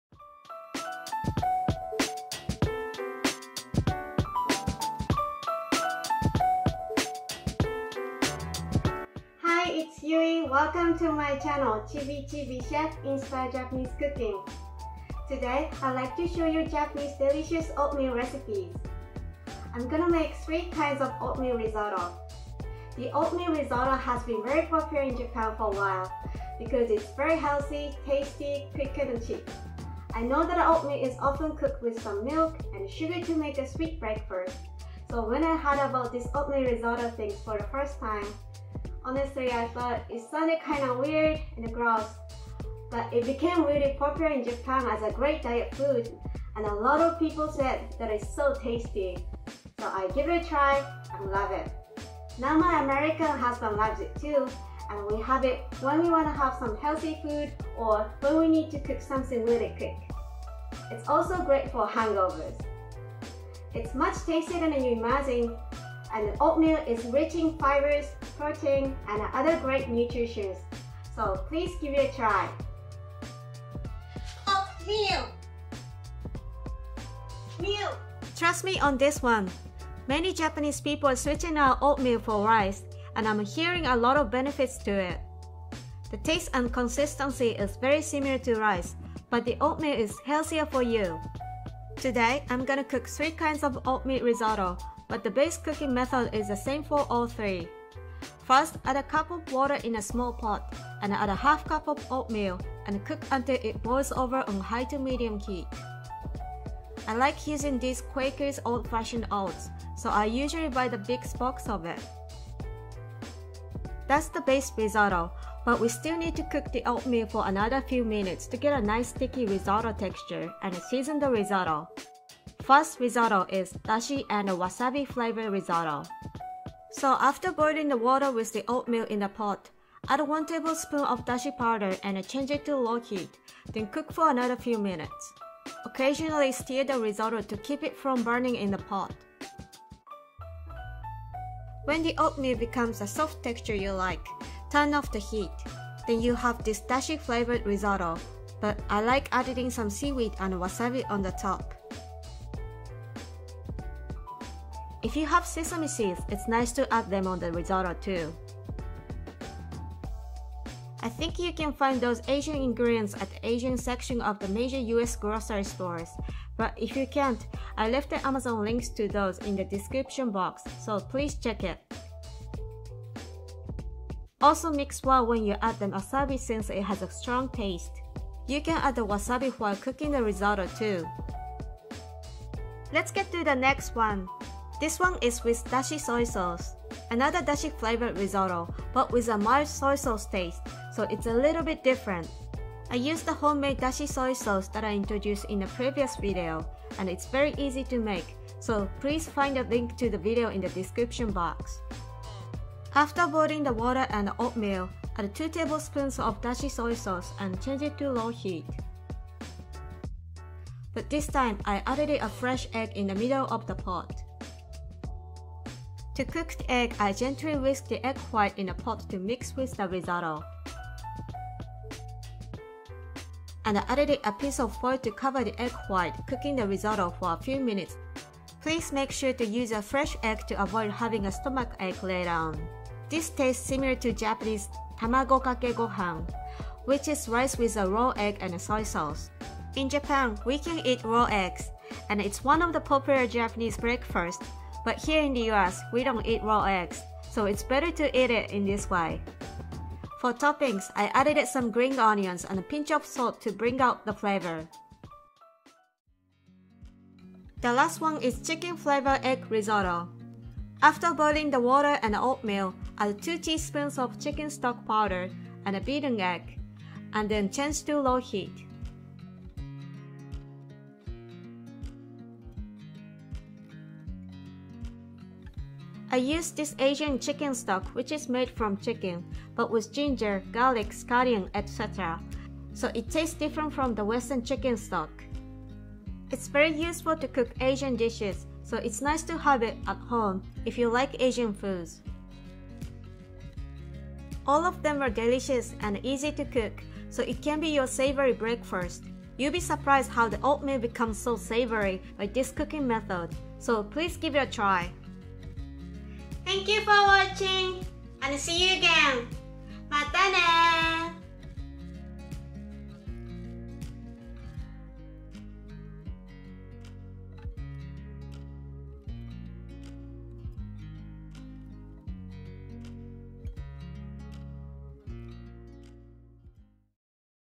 Healthy Japanese Breakfast | Quick & Tasty Rice-like Oatmeal Risotto
It’s worth trying if you have oatmeal at home or if you wanna make your breakfast healthier! It’ll taste totally different from your usual sweet oatmeal breakfast, and you’ll be surprised how the oatmeal Japanese risotto is so tasty.

- [Authentic Japanese RICH&JUICY TERIYAKI BURGER]

Recipes
*All measurements are in US standard format
[Ingredients for 1 portion]
Base risotto:
1 cup          Water
1/2 cup      Oatmeal

Dashi & Wasabi Risotto:
1 tbsp        Dashi powder (See the amazon links below for details)
To taste     Seaweed
To taste     Wasabi

Dashi Soy Sauce Risotto:
1                 Fresh egg

Chicken Flavor Egg Risotto:
2 tsp          Asian chicken stock powder (See the amazon links below for details)
1                 Egg
To taste    Sesame seeds

[Amazon US links for the ingredients]
Dashi powder (Bonito mix)
Dashi powder (Vegan Kombu)
Seaweed ( You can use Korean seaweed too!)
Sesame seeds
Asian chicken stock powder